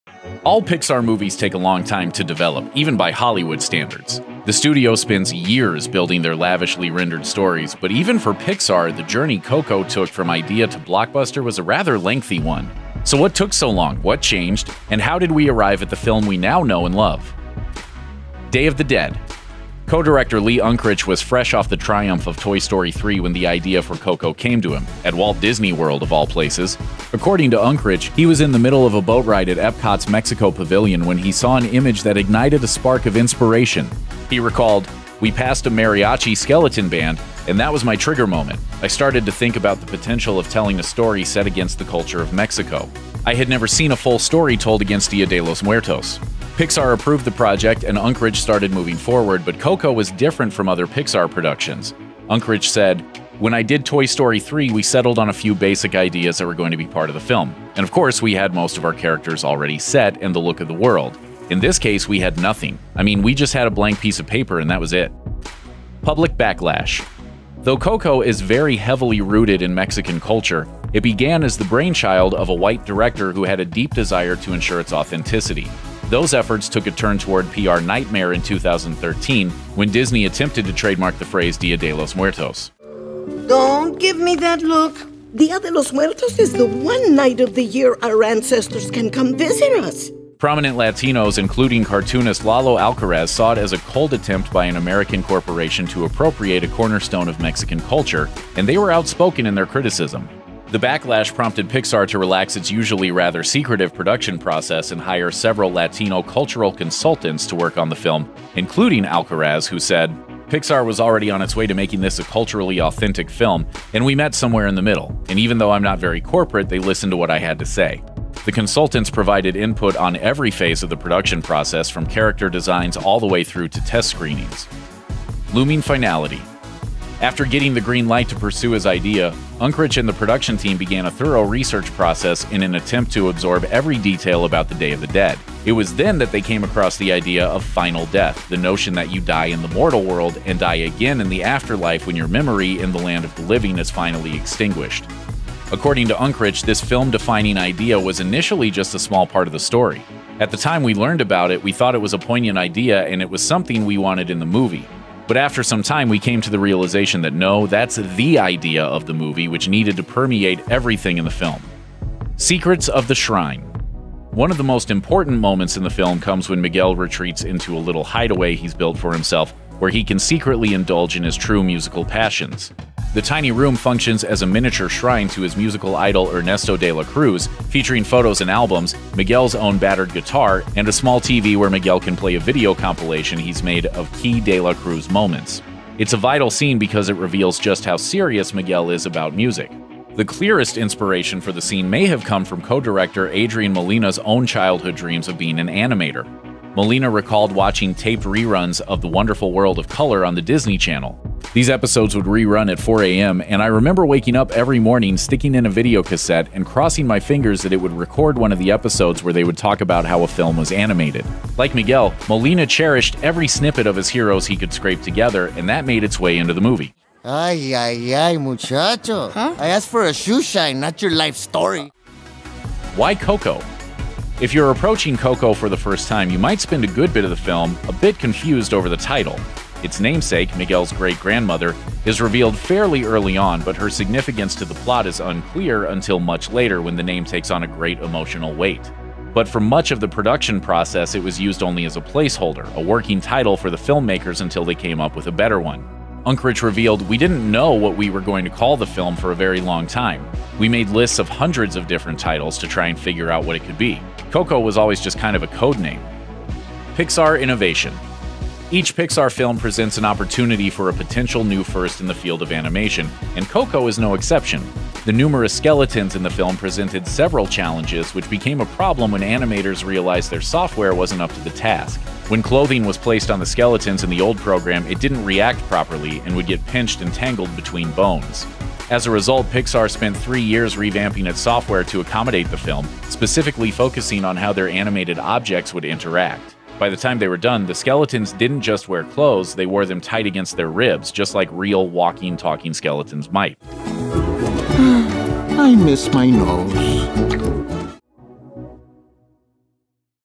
The Untold Truth Of Coco
Coco has proven once again that Pixar knows how to tell an emotional story that can pull on the heartstrings of kids and adults alike. However, there were a lot of hiccups during production that could have spelled disaster for the film, as it was originally supposed to focus on an American family who attended Mexico’s Dia de Los Muertos, and Disney really stepped on some toes by trying to trademark the name of the holiday. Thankfully, it all worked out in the end, and the film was a hit with critics and audiences. Let’s take a look at the untold truth of Coco.

Day of the Dead | 0:00
Public backlash | 1:06
Looming finality | 2:05
Secrets of the shrine | 2:42
Why Coco? | 3:42
Pixar innovation | 4:17